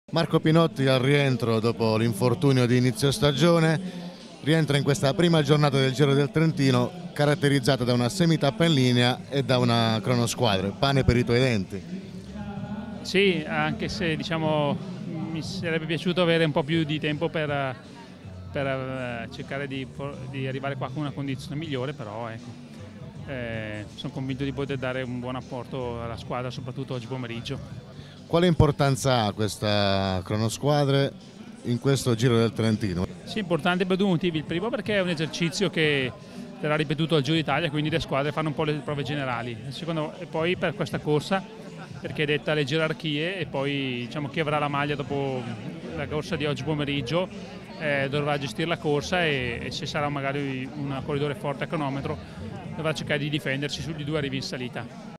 Giro del Trentino 2013 - Marco Pinotti before the first half-stage
Al rientro dopo l'infortunio che ne ha compromesso la prima parte della stagione, Marco Pinotti attende la cronosquadre per affinare i meccanismi con i compagni della BMC in vista del Giro d'Italia / Back to race after the injury suffered at Le Tour Méditerranéen, Marco Pinotti stays focused on the TTT to improve the BMC team's mechanisms in view of the Giro d'Italia